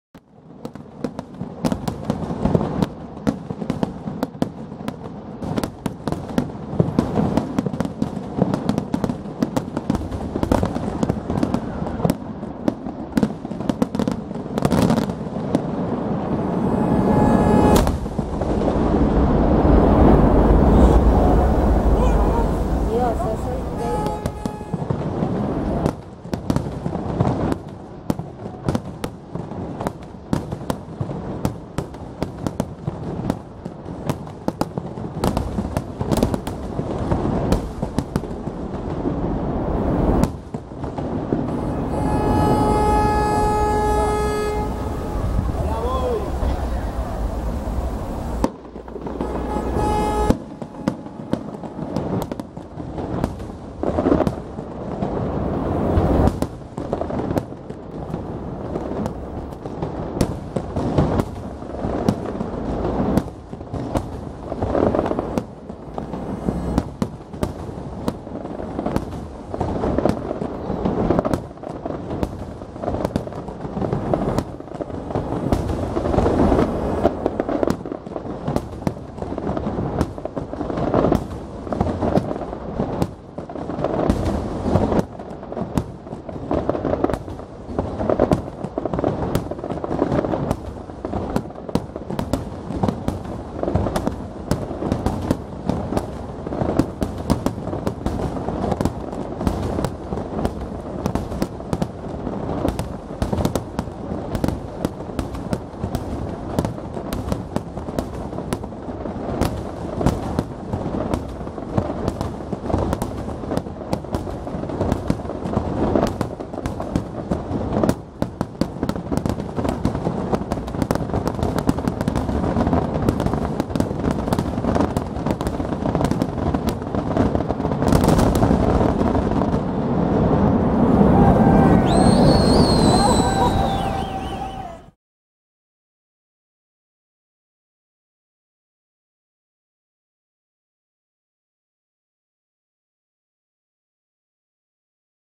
FUEGOS ARTIFICIALES-Pamplona 2012
Grabación de los fuegos artificiales ya finalizando. 09/07/2012